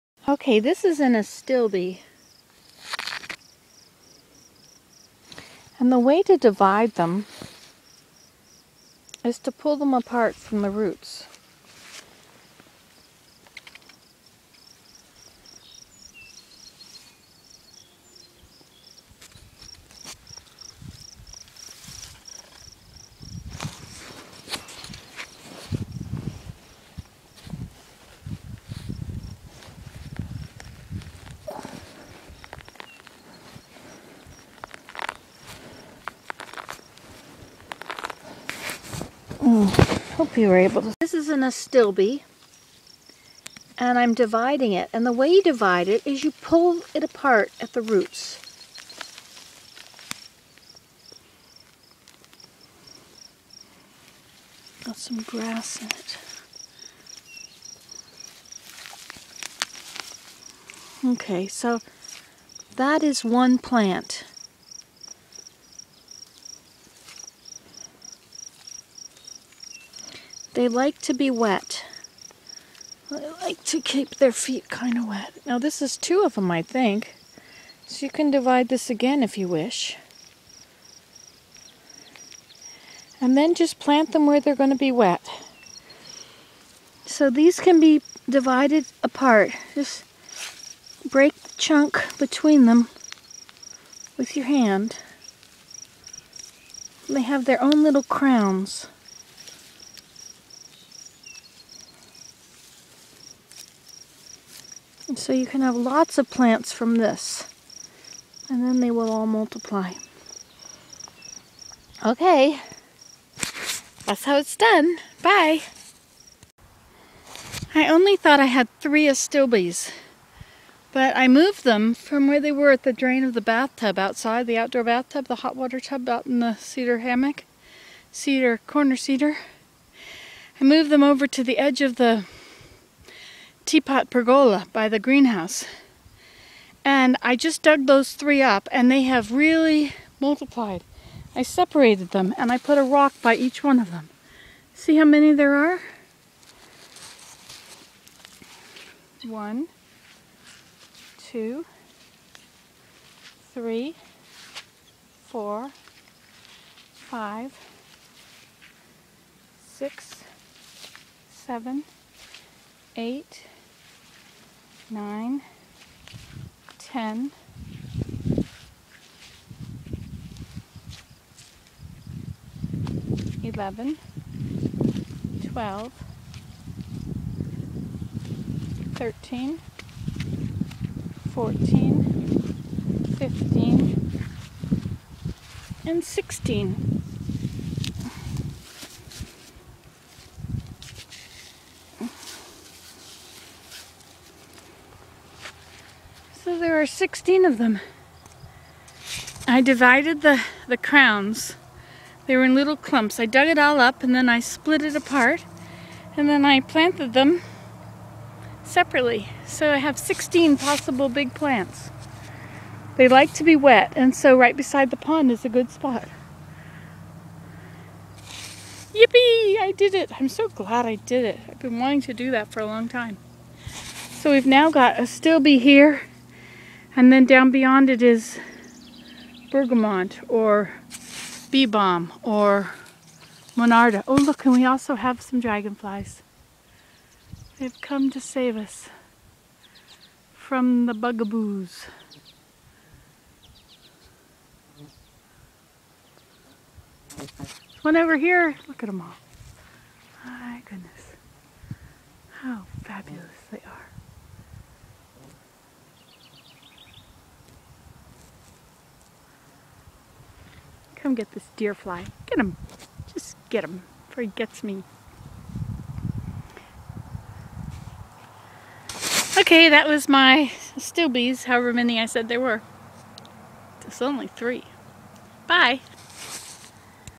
Nancy Today: How to propagate astilbe plants
I have dug up three astilbe plants where they weren't growing very well.  They weren't getting bigger.  Divide the crowns with your fingers, then plant each new plant out.  Three not very good plants made 16 new plants.